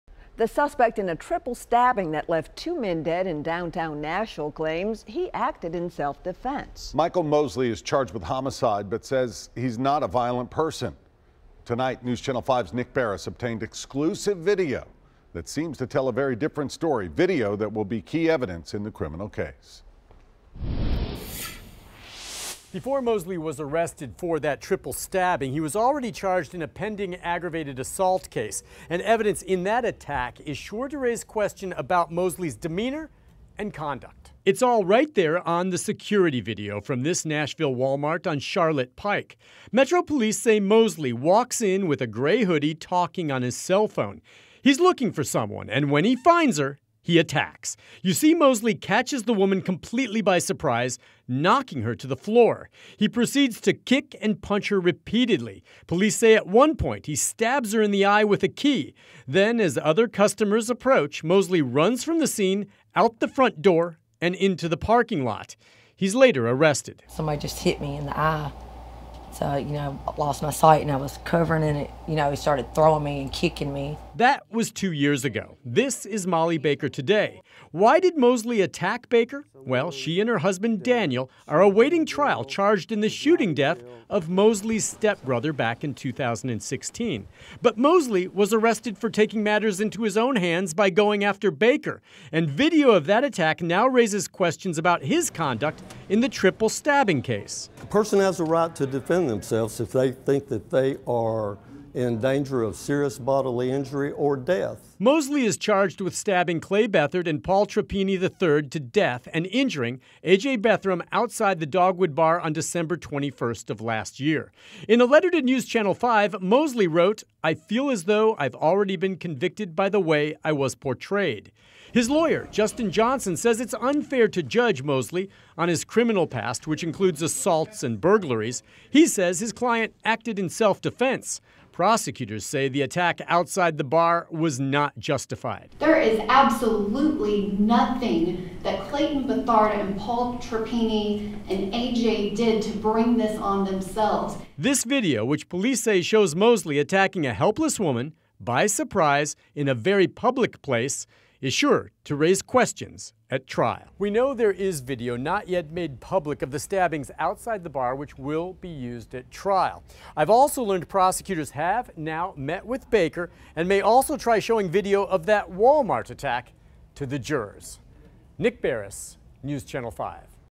Police: Security video shows Michael Mosley attacking woman inside Walmart
Michael Mosley, the suspect in a triple stabbing that left two men dead in downtown Nashville claims he acted in self-defense. But now exclusive video seems to tell a very different story — video that will be key evidence in the criminal case.